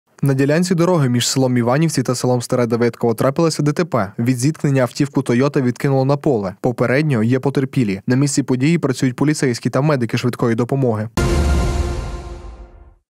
Моторошна ДТП
На ділянці дороги між с. Іванівці та с. Старе Давидково трапилася ДТП. Від зіткнення автівку Toyota викинуло на поле. Попередньо, є потерпілі. На місці події працюють поліцейські та медики швидкої допомоги.